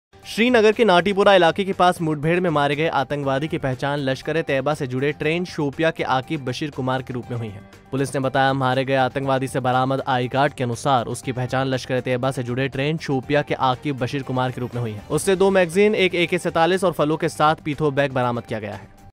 नातिपोरा श्रीनगर में एनकाउंटर
नातिपोरा श्रीनगर में एनकाउंटर, लश्कर तोयबा का आतंकवादी मारा गया